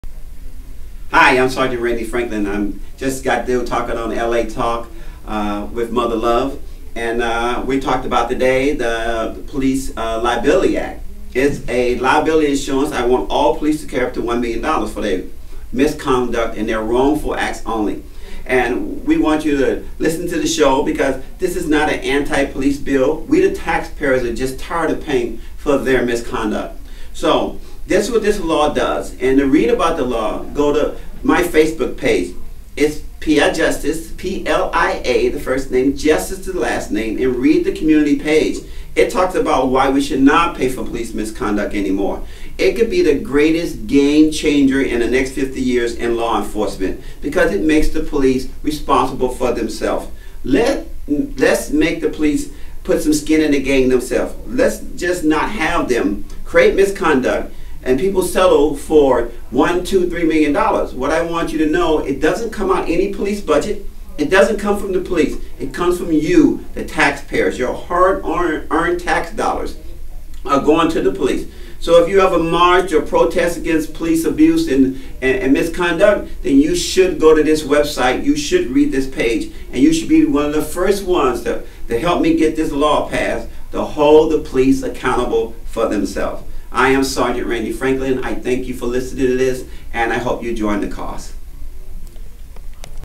Randy Franklin
Hi Cyber Babies. Today on LATalkRadio Mother Love talks with Randy Franklin, a 30 year LAPD officer about Police Liability Insurance Act. Community council for police Accountability. Misconduct Let's not condone it let's not pay for it. This and so much more.